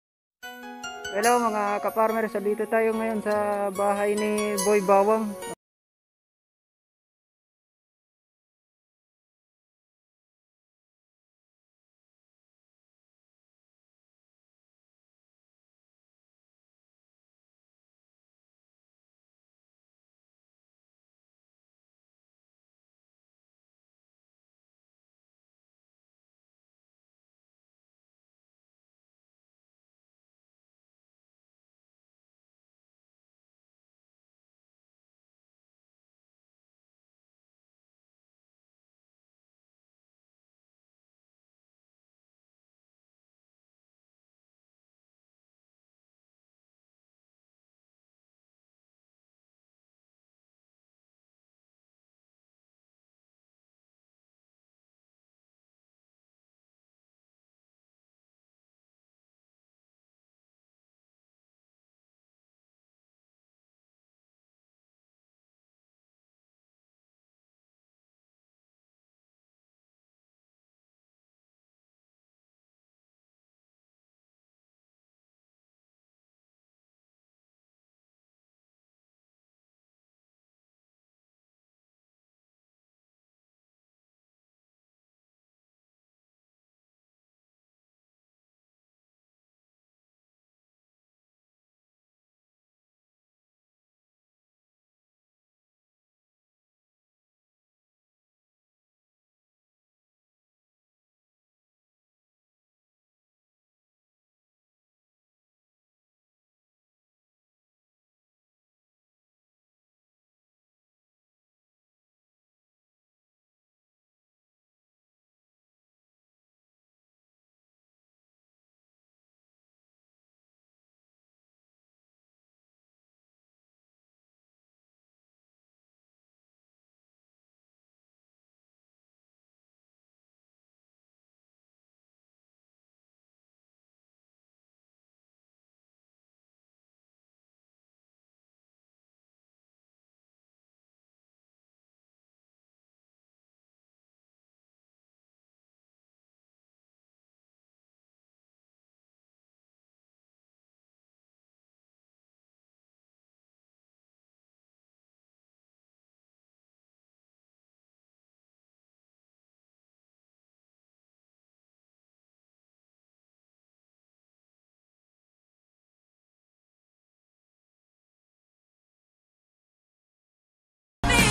Merenda para sa mga bata...may kunting blessings lang😁
napaka saya sa pakiramdam ang makapag bigay saya lalo na sa mga bata